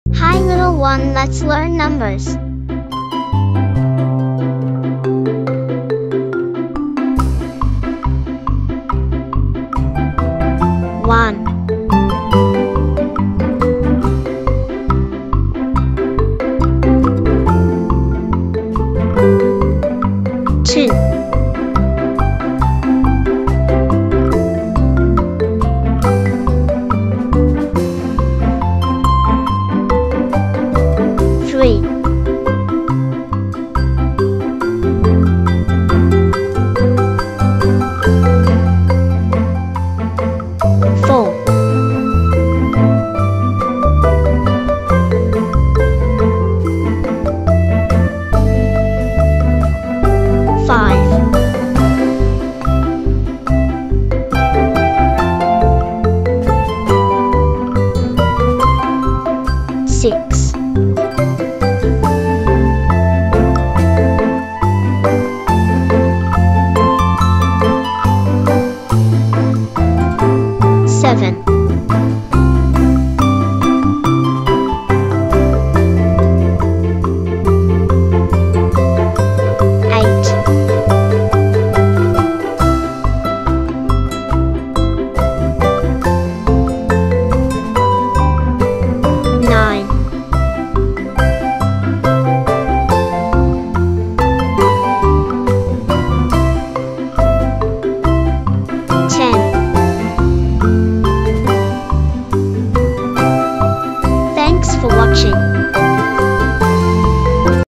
"Learn Numbers 1 to 10 for Kids | Fun Counting & Number Learning Video | Preschool & Kindergarten"
"Welcome to our fun and interactive numbers learning video for kids! In this video, children will learn numbers 1 to 10 with colorful animations, cheerful music, and easy counting activities. Perfect for toddlers, preschoolers, and kindergarten kids, this video makes learning numbers fun and engaging with playful animations and interactive cues.

Young learners will enjoy counting objects, hearing each number out loud, and participating in the 'Mystery Number' game to reinforce their understanding. This video is designed to build a strong foundation in counting and basic math skills. Let’s make learning fun and start counting together!"

Preschool numbers
Kindergarten learning
Fun learning for toddlers
Early childhood education
Number learning for kids